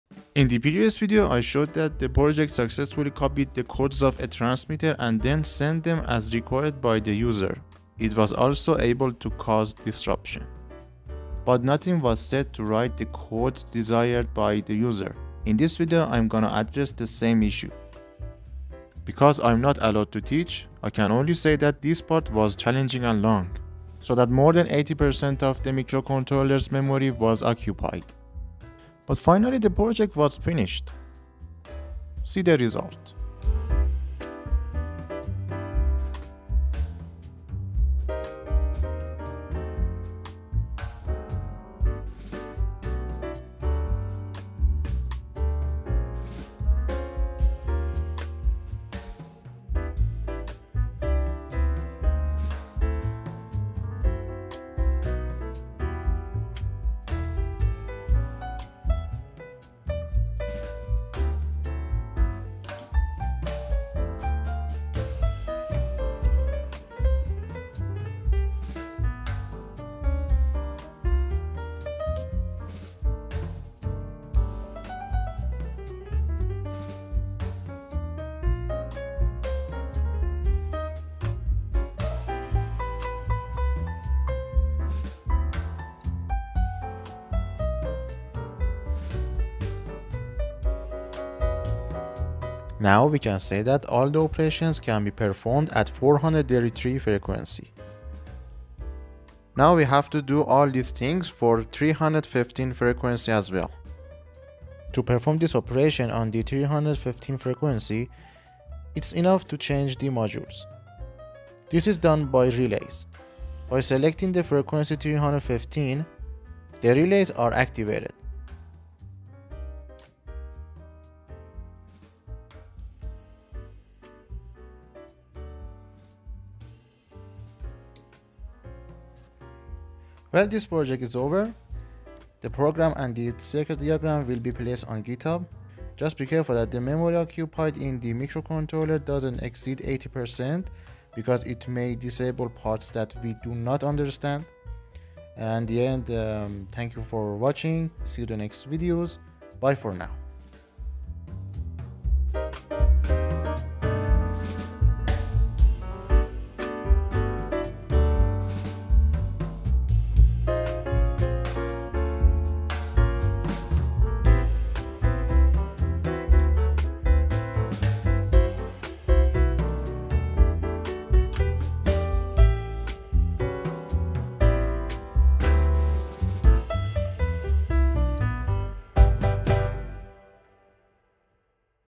RF Hacker - Part 2 - 433 & 315MHz Using an arduino
Finally, part 2 of RF Hacker is ready. Sorry if it's late. In this section, I completed writing the code desired by the user and explained how to replace the modules. I hope you enjoy this short video.
🔴 This video was made with the help of legal tools available in the market, and it is only educational, and I had no other intention to make it.